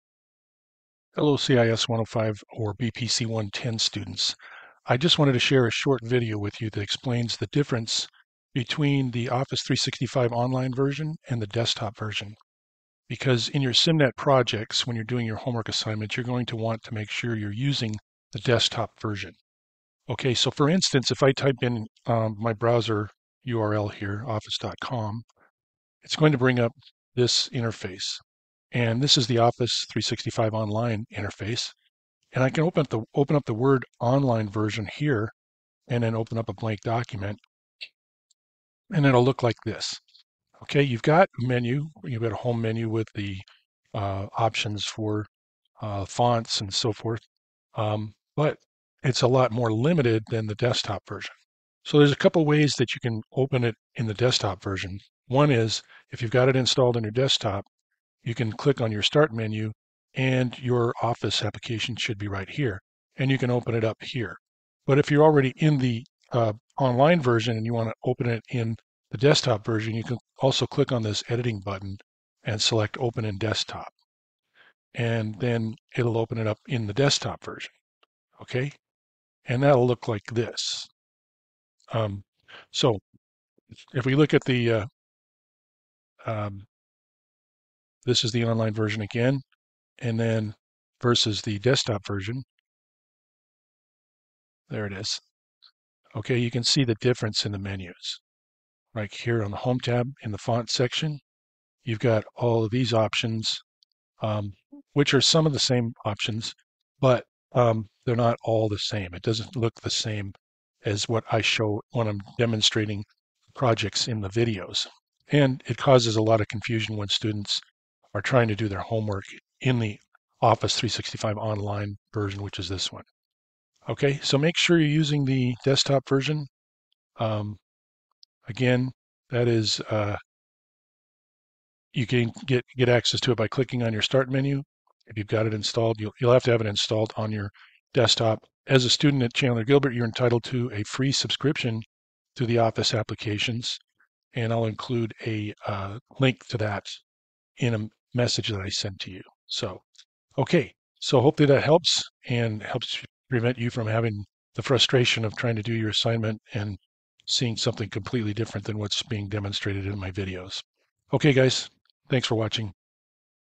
Office 365 Online VS MS Office Desktop
Differences between Office 365 Online version and the MS Office Desktop version explained.